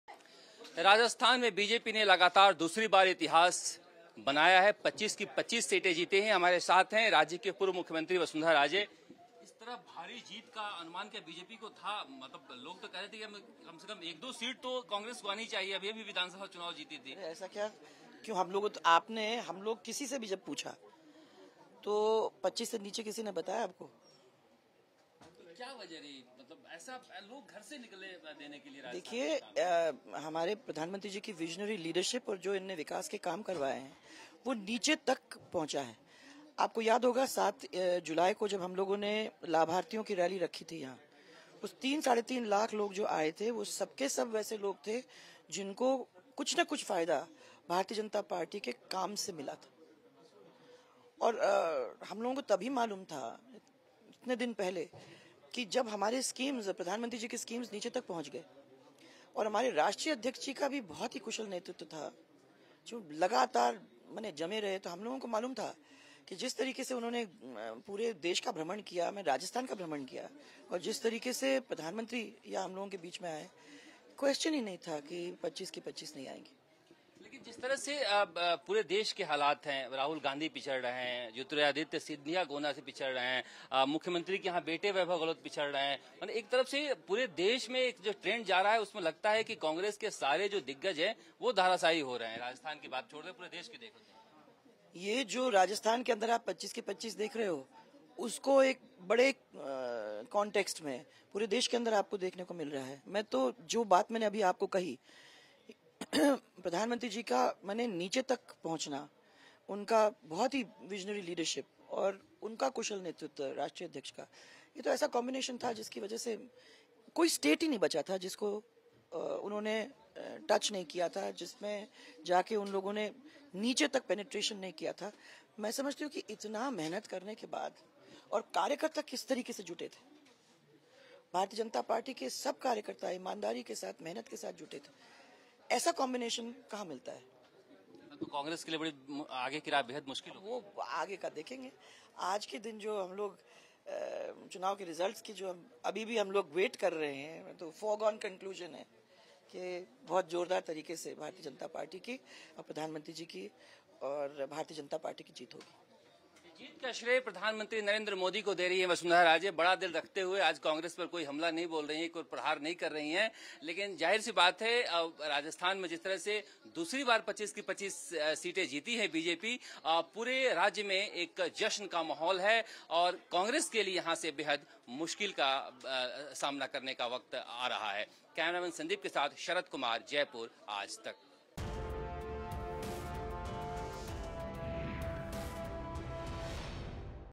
JAIPUR-BJP की जीत पर VASUNDHARA RAJE का बड़ा बयान !
LOKSABHA ELECTION RESULT- MODI की वजह से हमें राजस्थान में इतनी बड़ी जीत मिली- VASUNDHARA RAJE